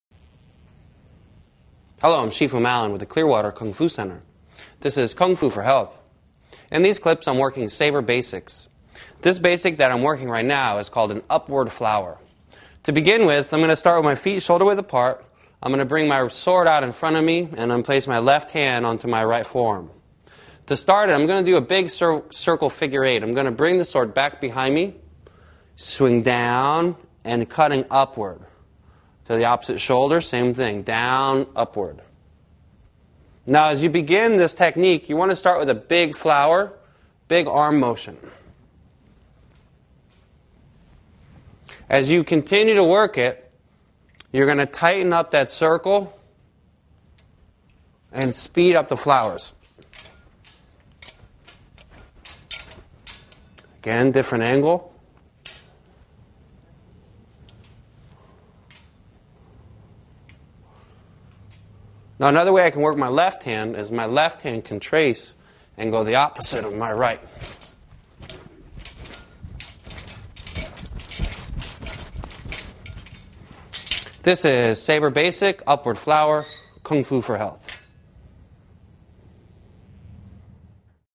Kung Fu Saber Basics: Upward Flower
Kung Fu Saber Basics: Upward Flower. Part of the series: Kung Fu Saber Basics. When performing kung fu with a saber, start with the feet shoulder width apart while performing figure eights to execute the upward flower sword technique. Practice this potent attack with tips from a professional kung fu instructor in this free martial arts video.